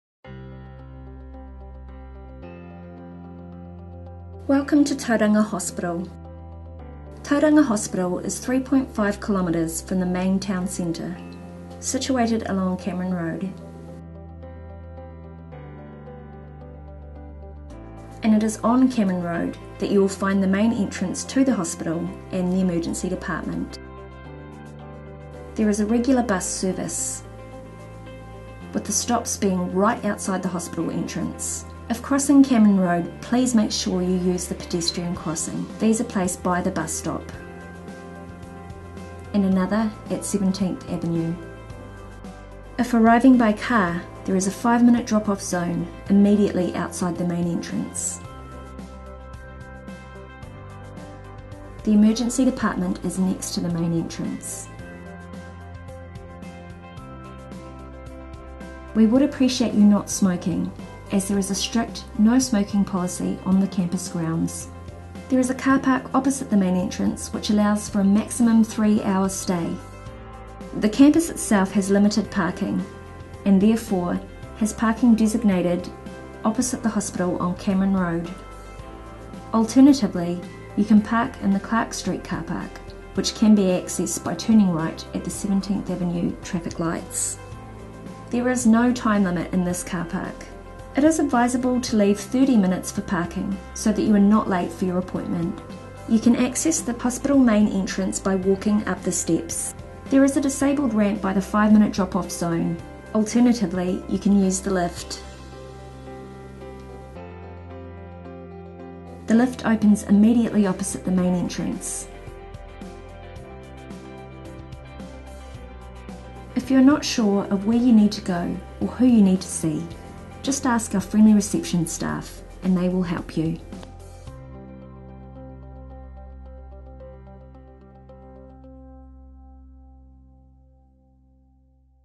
BoPDHB - Tauranga Hospital Parking
Welcome to Tauranga Hospital - Directions and parking advice when visiting Tauranga Hospital